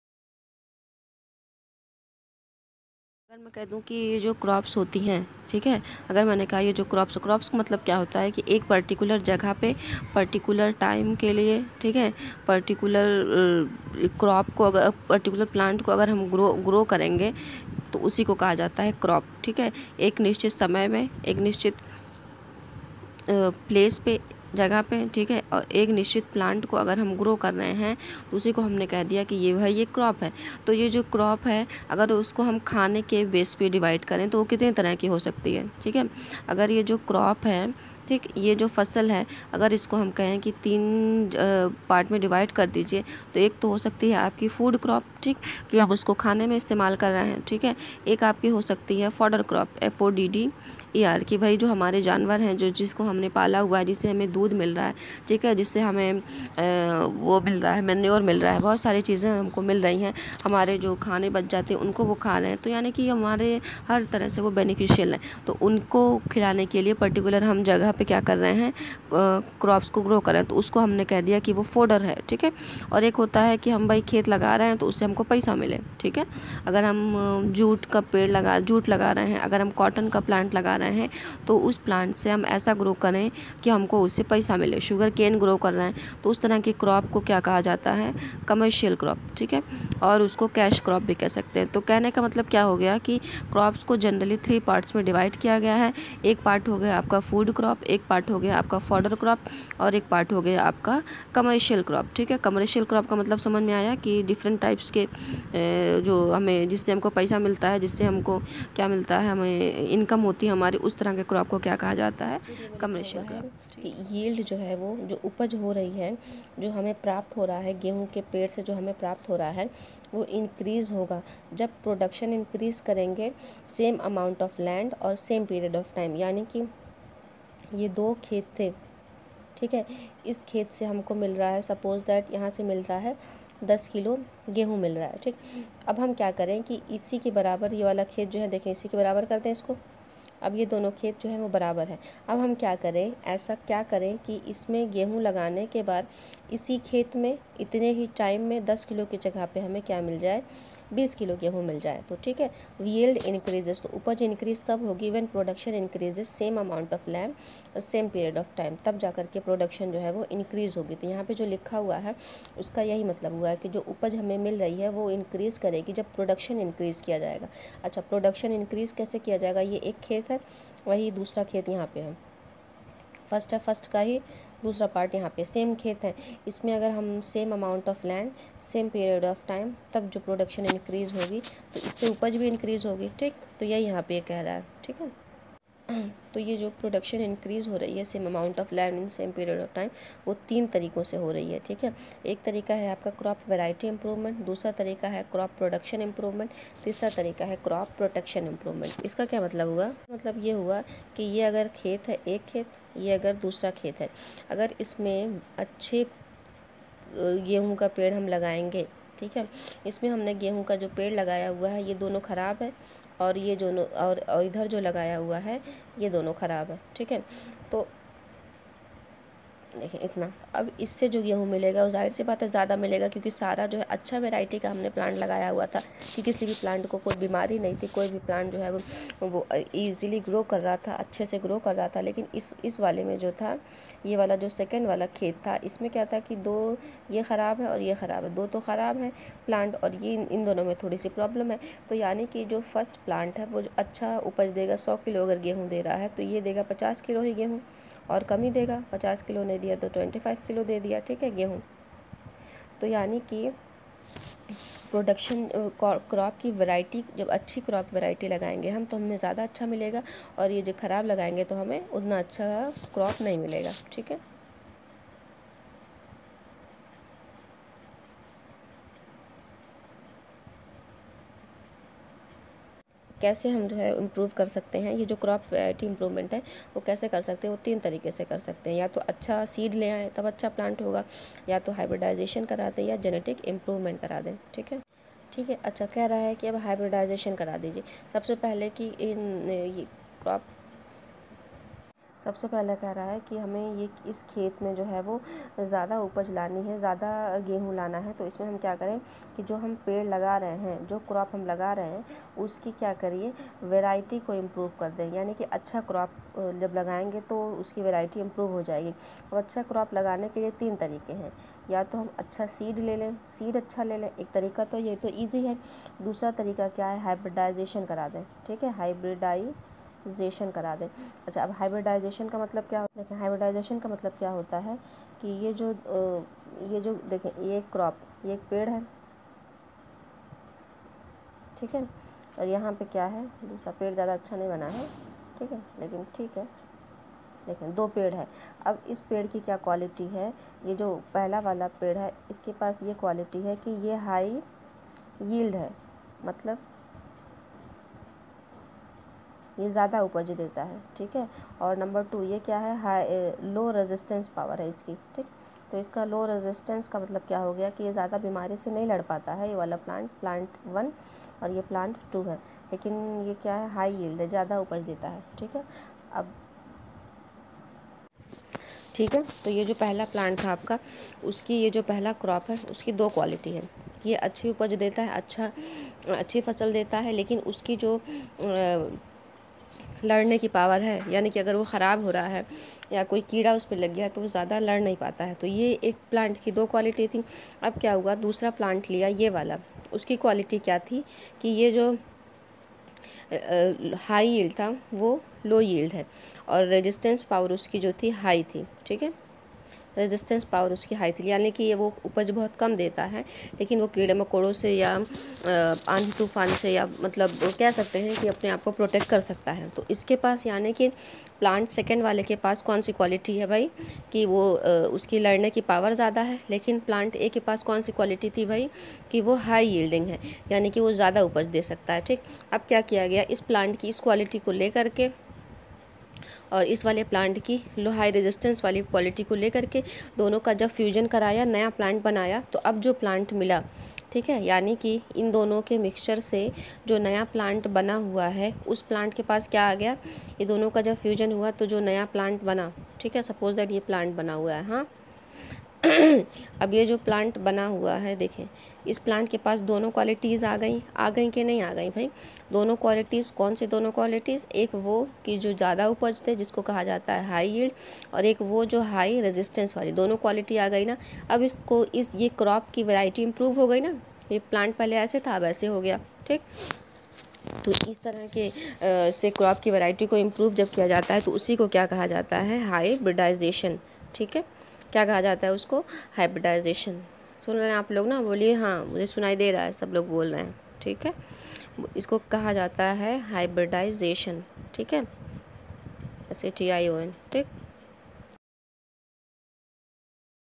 BIOLOGY # NCERTCBSE#2021
1. improvement in food resources
2. different types of crop
क्रॉप्स बेस्ट ऑन सीजन रबी क्रॉप खरीफ क्रॉप
3. different types of crops on the basis of food fodder and Cash crop
3. improvement in crop yield
4. all the basics related to crop
5. important chapter according to NCERT